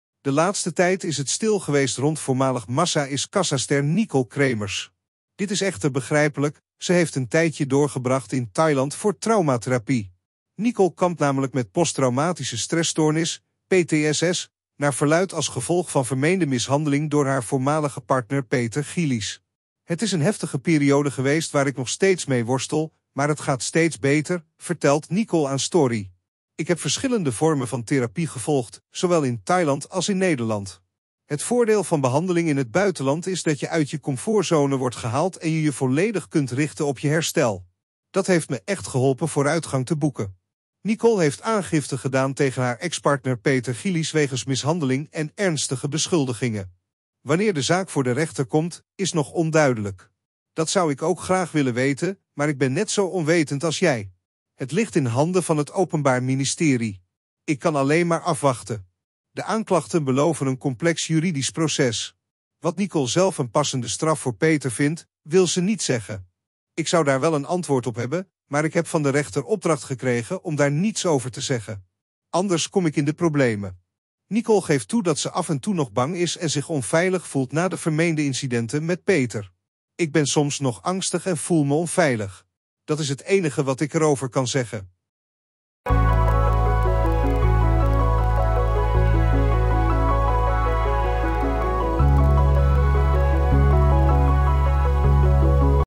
Nicol Kremers doorbreekt stilte na traumatische ervaringen en juridische strijd
Bekijk het recente interview met Nicol Kremers, voormalig Massa is Kassa-ster, waar ze openhartig spreekt over haar traumatische ervaringen en de juridische strijd tegen ex-partner Peter Gillis. Ontdek hoe ze in Thailand traumatherapie heeft ondergaan en welke vooruitgang ze heeft geboekt. Nicol heeft aangifte gedaan wegens mishandeling, maar de datum van de rechtszaak blijft onduidelijk. Volg het complexe juridische proces en haar reacties op deze emotionele reis.